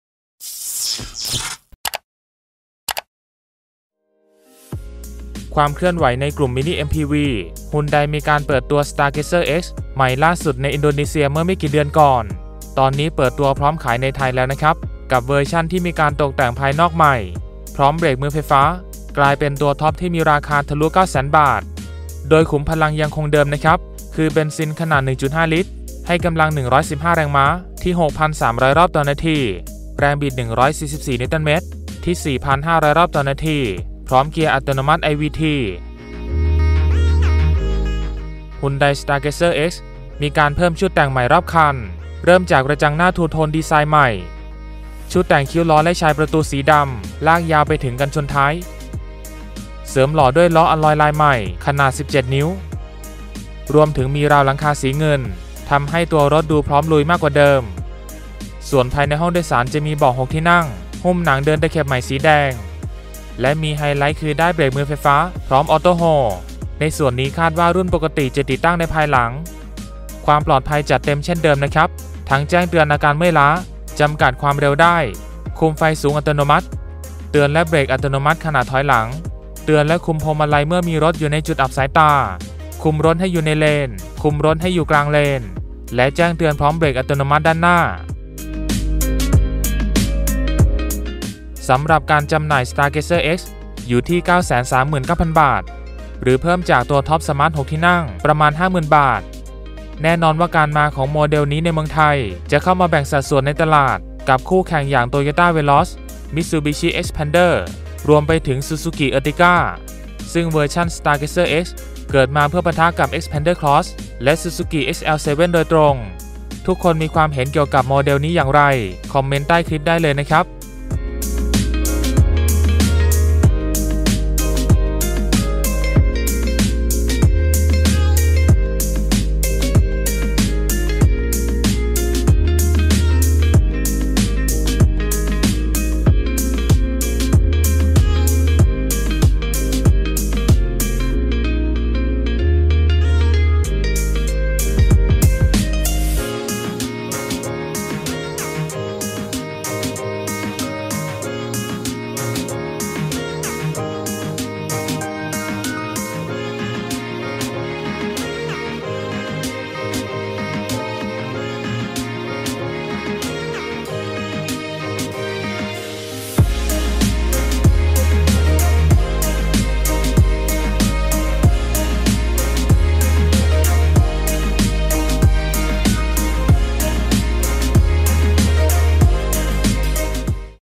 มาแล้ว! เปิดตัว Hyundai Stagazer X รุ่นเพิ่มชุดแต่งใหม่รอบคัน..ราคาในไทย 9.39 แสนบาท!
ติดตามข่าวสารและพูดคุยได้บนแฟนเพจ